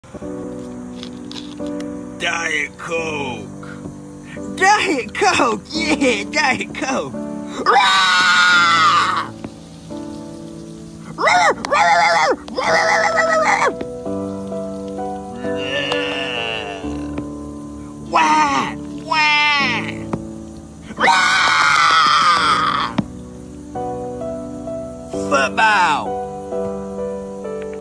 diet coke
@nifreffa on tictick

i found uploaded on some cringeposting page and but i have a laugh everytime i watch this, and it is simply easier for me to accept that i, qtg, simply have a more developed and sophisticated take on comedy than the philistine cromagnon which surround me, and if my opinions on shaking diet coke and screaming become too unpopular, i can just hide under 3 layers of irony,; simply amazing, jester's privilege at work